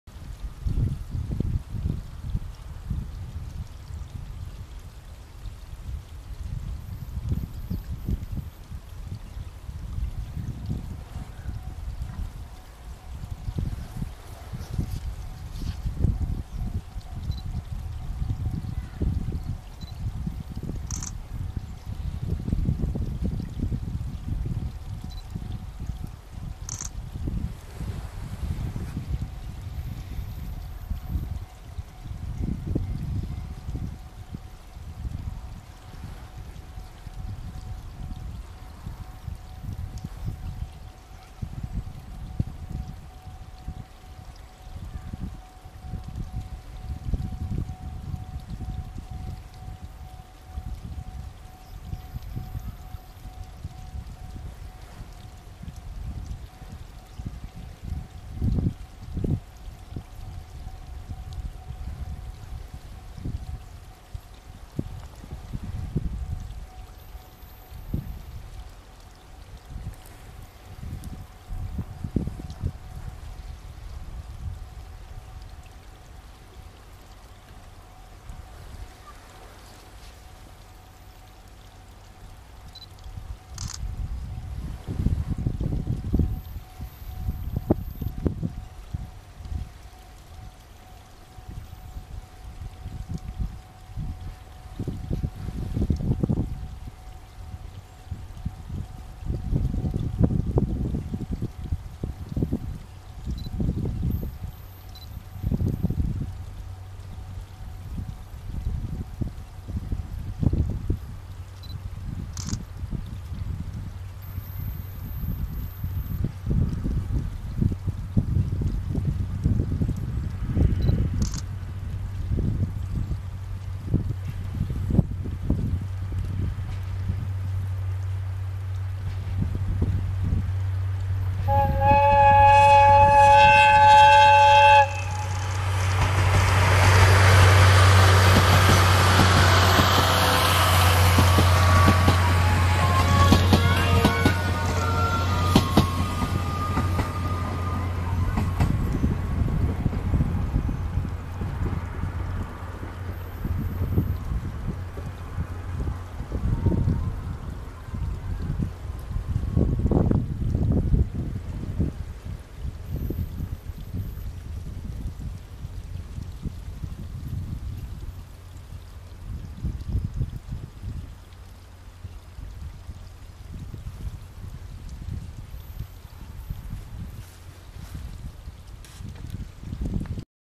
東武鉄道 鬼怒川線 DL大樹3号 大谷向～大桑間通過
東武鉄道 鬼怒川線 DL大樹3号の大谷向～大桑間、
倉ヶ崎SL花畑付近通過の模様です（2022/02/26）